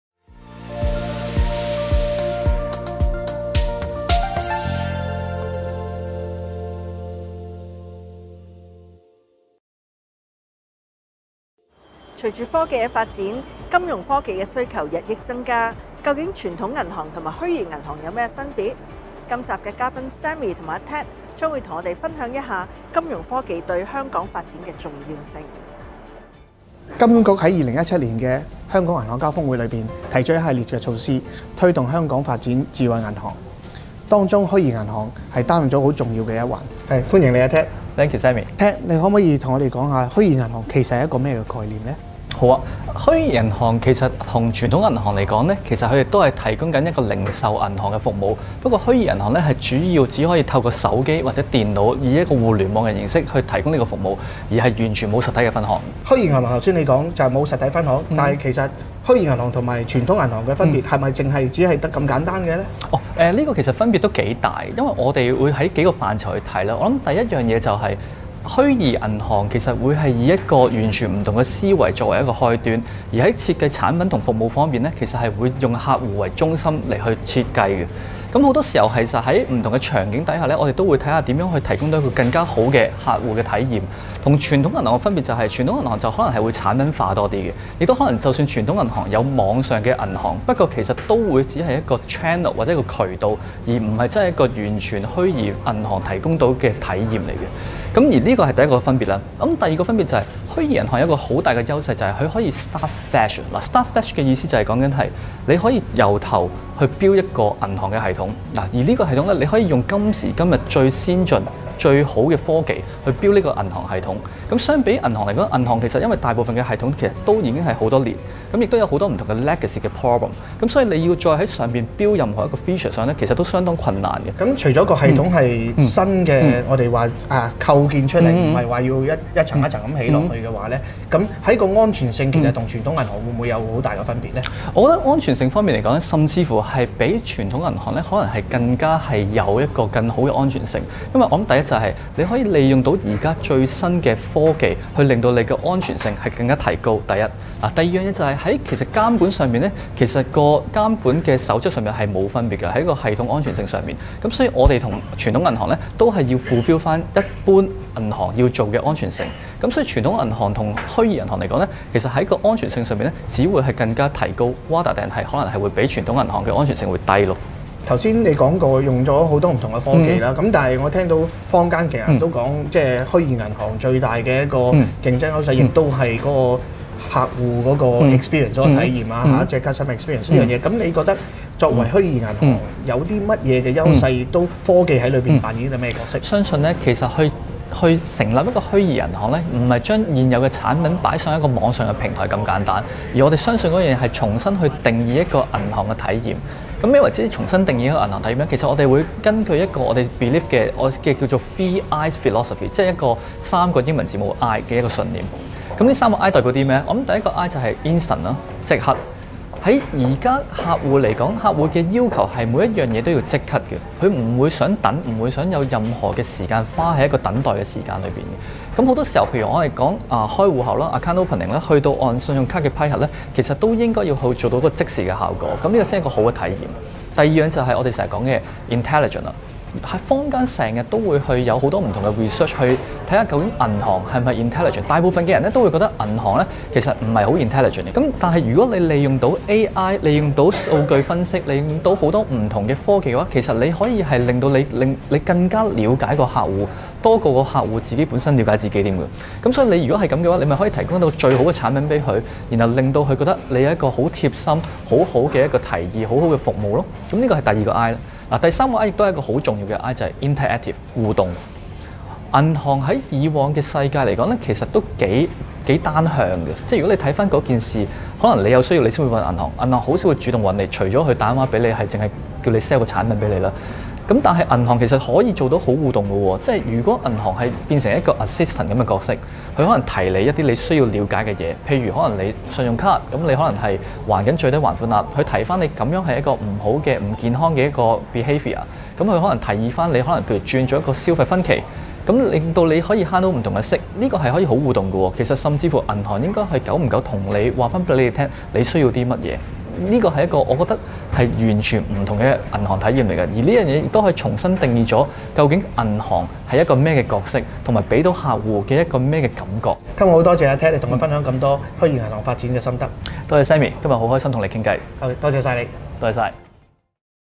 百談科技#002 - 認識更多「虛擬銀行」
虛擬銀行在今年開始正或在本地消費市場出現，一個一切只在手機或電腦上進行運作的銀行服務，跟傳統有分行網絡的銀行服務到底最大分別在哪？虛擬銀行會否取代傳統銀行？還是相輔相成？《百談科技》第二集，就跟大家解釋一下虛擬銀行的特點同優勢。